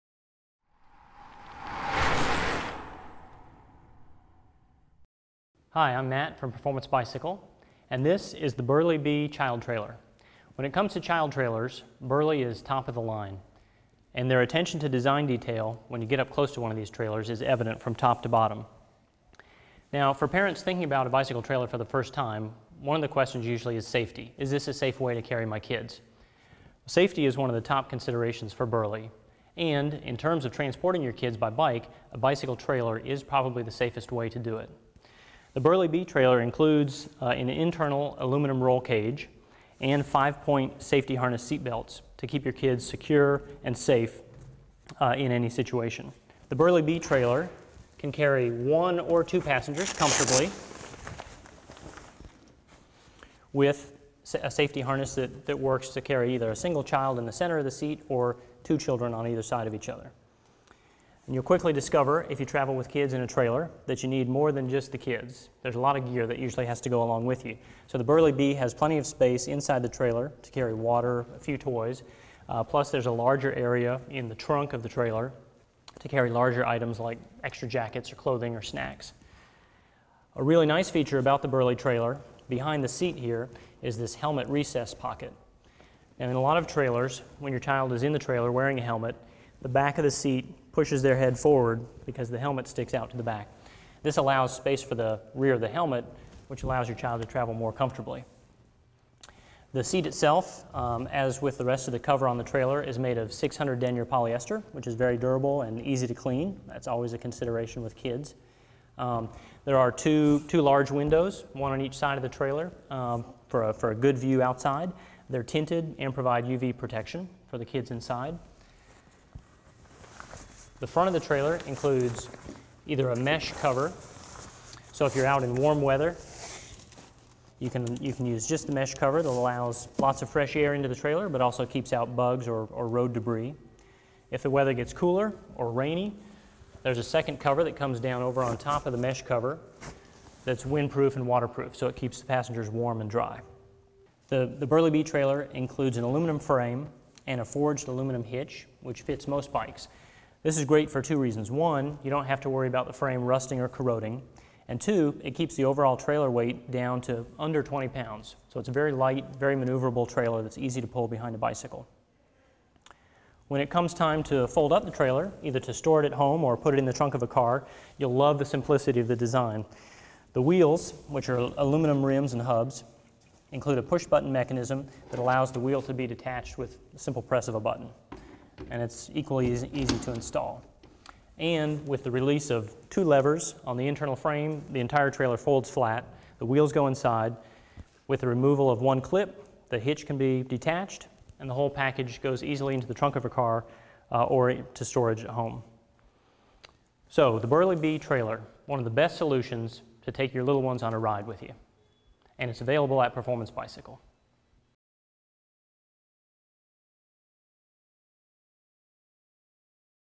Burley Bee Child Trailer Review from Performance Bicycle
Great to bring the kids along on your town rides, let your dog ride along with you, or have some extra storage space on those grocery trips, the Burley Bee Child Trailer from Performance Bicycle is sturdy dependable and fun trailer that puts safety first on your daily rides.